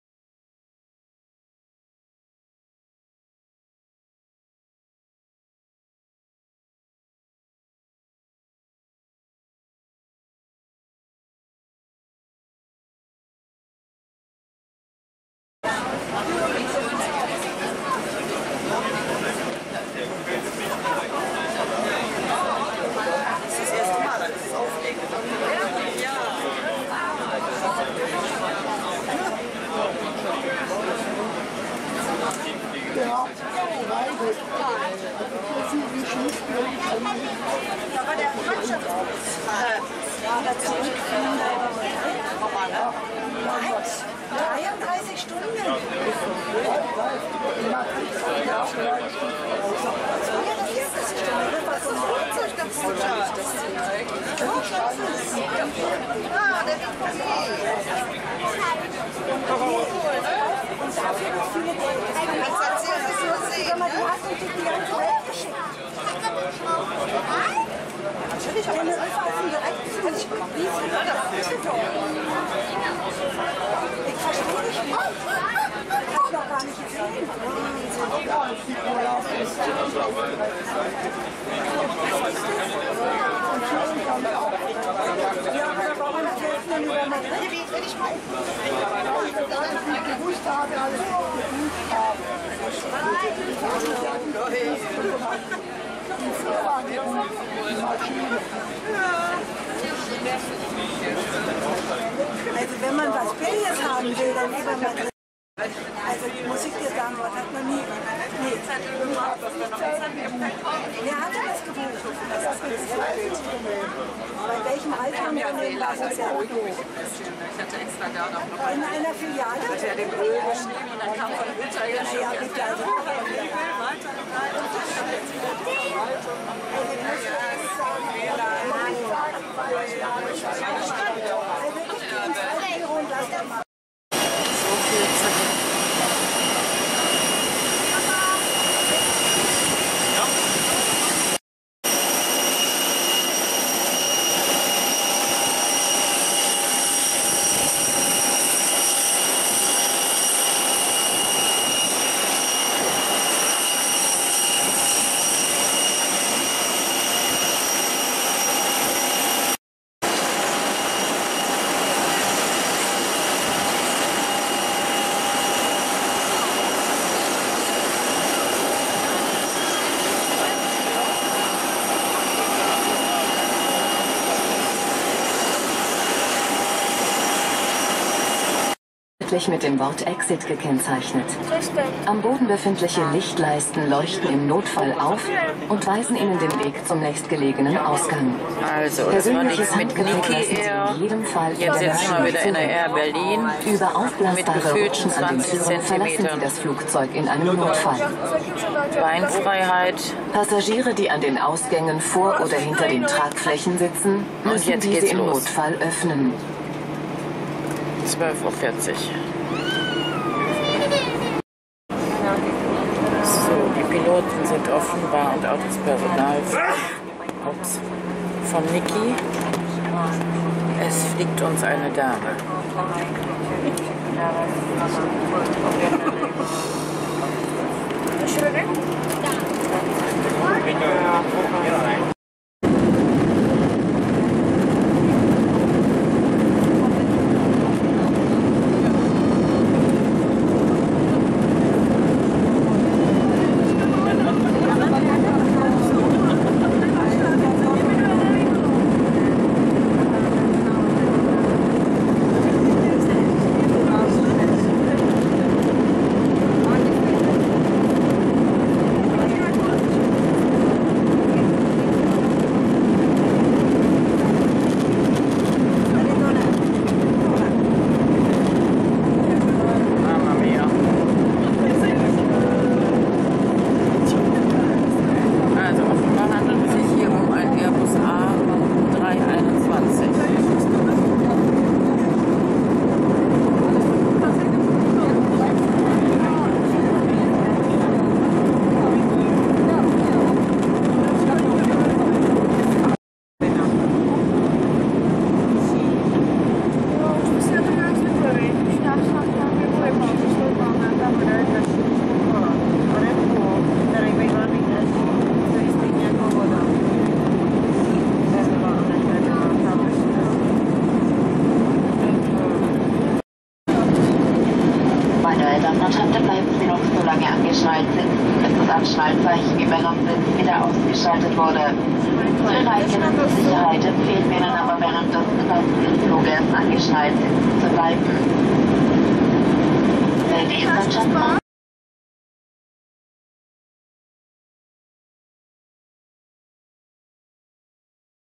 Berlin Flug Düsseldorf - Teneriffa Süd , Flight DUS - TFS to Tenerife South
Flug HG2050 Flugfeld und Start in Düsseldorf. Leider nicht mit Niki wie vorgesehen sondern einer AirBerlin Maschine.

Flight HG2050 On airfield and take-off in Düsseldorf. Not Niki as planned but in an Air Berlin aircraft.

Video except print out of flightradar by and Copyright Cosmopolitan Lady.